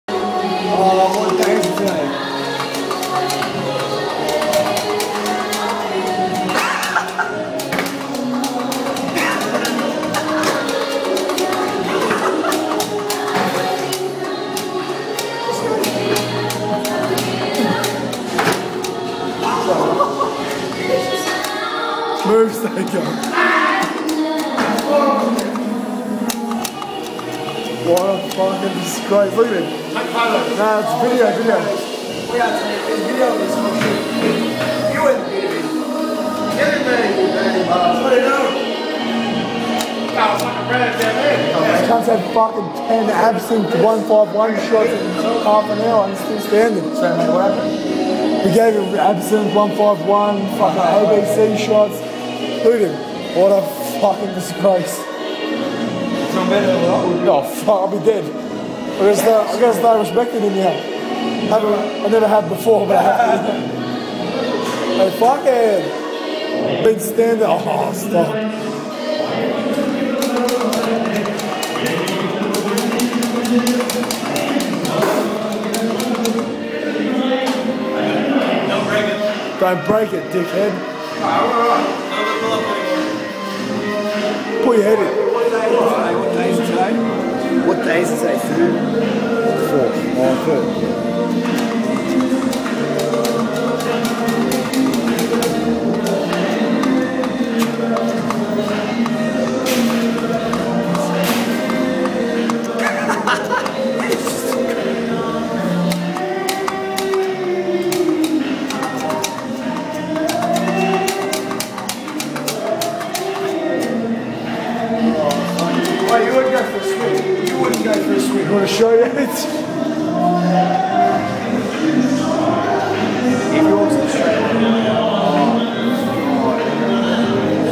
Wasted
This is what happens when you drink around 10 Bacardi 151/Absinthe/Chartreuse in the space of an hour or so.

Full credit to Yates for still being able to stand, although I didn't get to film him vomiting blood shortly after this video was filmed...